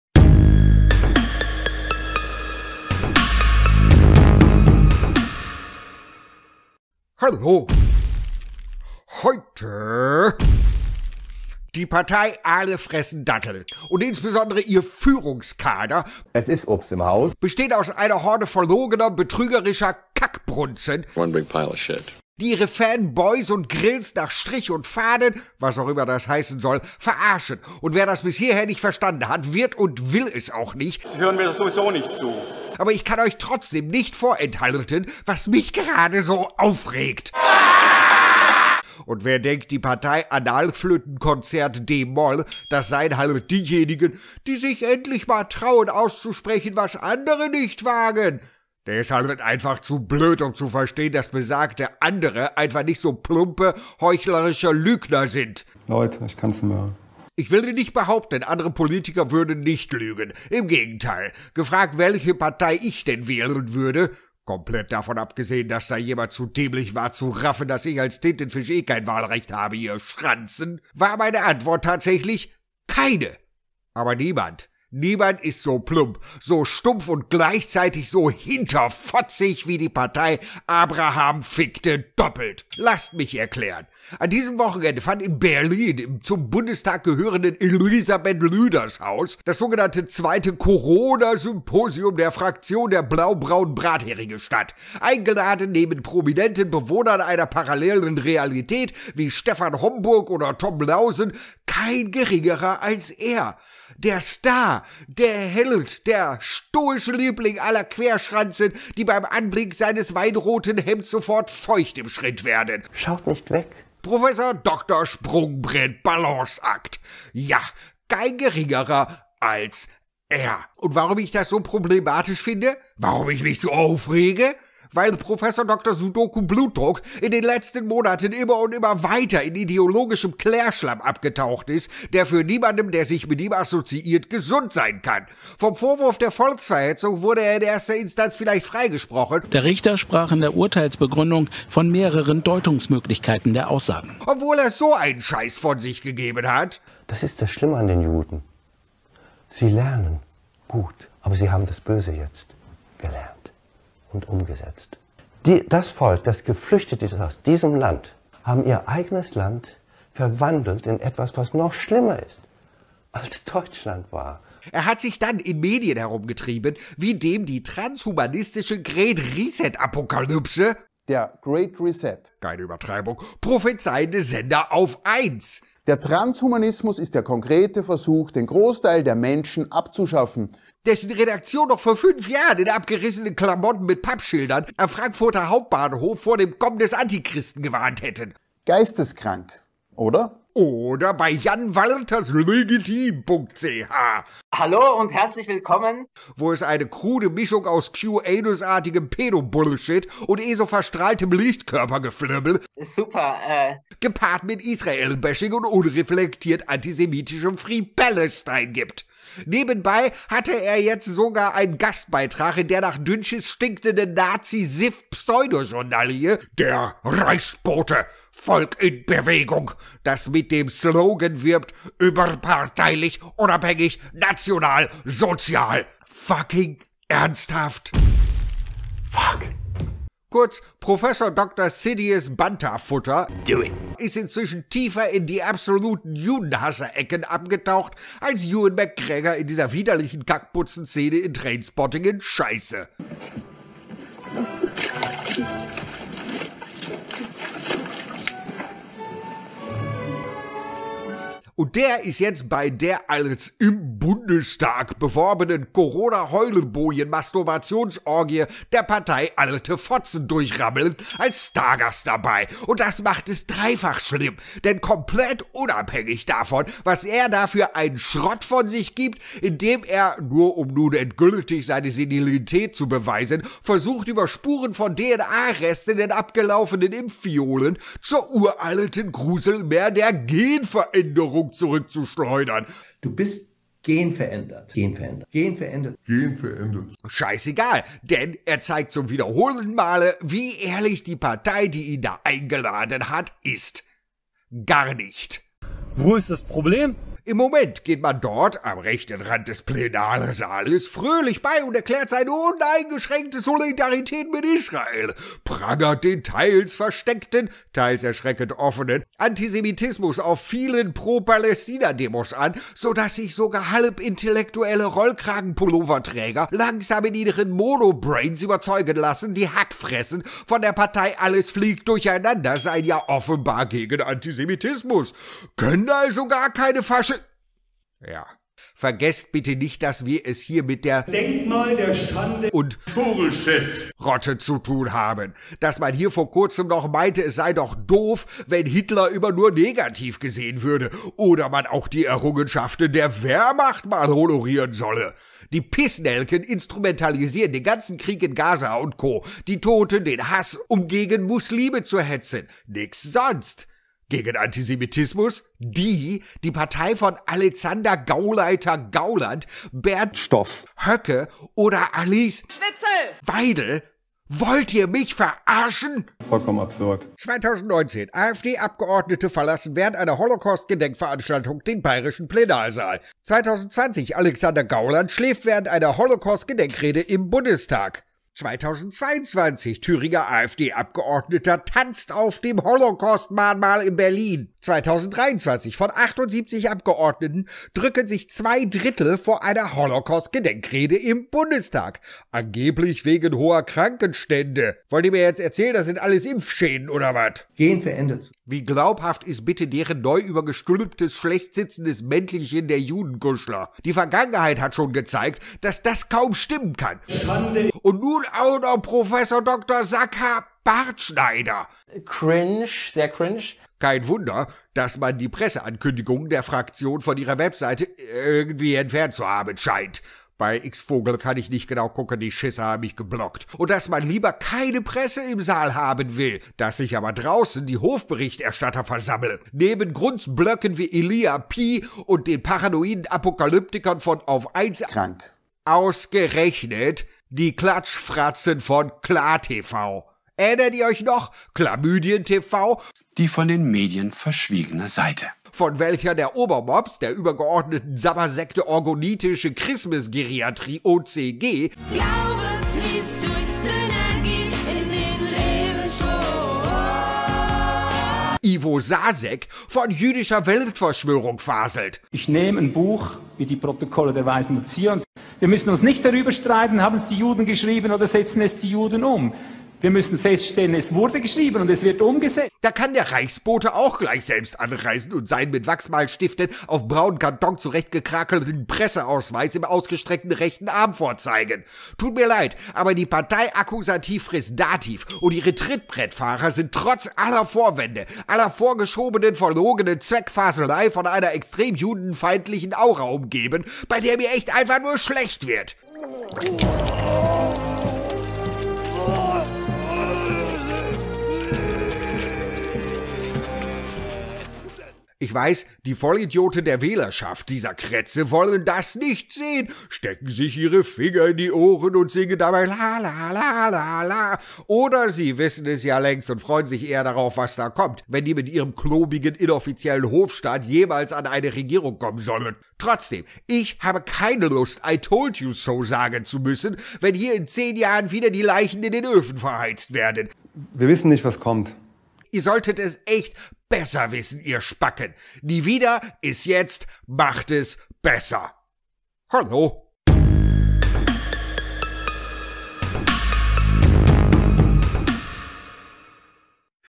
ERNSTHAFT? Ep. 29: Antisemiten im Bundestag?!
Am 11. und 12. November 2023 fand in den Räumen des Deutschen Bundestags das von der Fraktion der AfD ausgerichtete "2. Corona Symposium" statt. Einer der Gastredner: Sucharit Bhakdi, dessen Freispruch vom Vorwurf der Volksverhetzung nach wie vor recht problematisch vorkommt.

Seine Einladung wirft ein ganz, ganz komisches Licht auf diejenige Partei, die sich gerade so schön gegen Antisemitismus stark macht...

Quellen der Screenshots und weitere Bildquellen sind i. d. R. im Bild angegeben. Als Videos wurden u. a. verwendet: